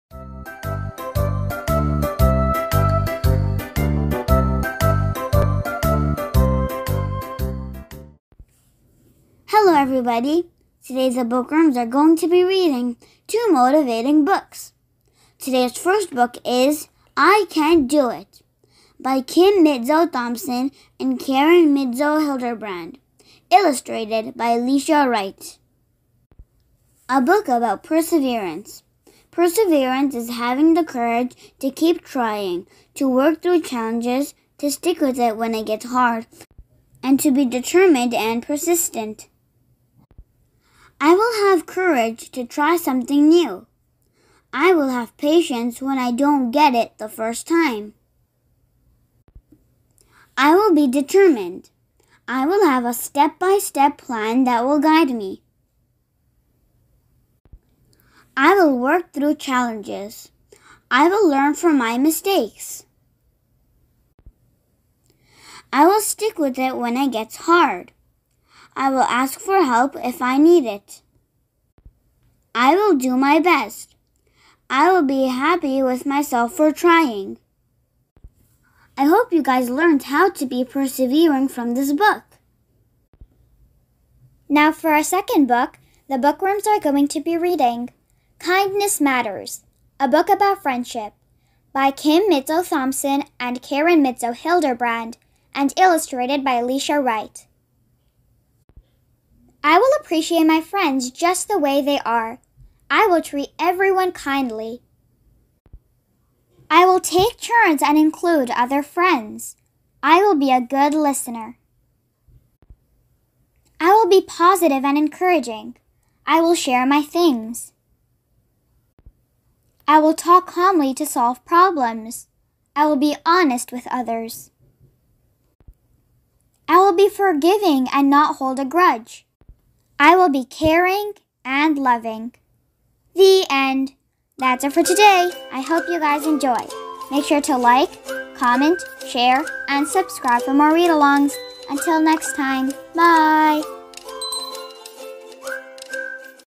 I can do it! and Kindness Matters! - By Kim Mitzo Thompson and Karen Mitzo Hilderbrand
Hello everybody, today the Bookworms have read, I can do it! and Kindness Matters- By Kim Mitzo Thompson and Karen Mitzo Hilderbrand!
Make sure to tell us which part of the book is your favourite!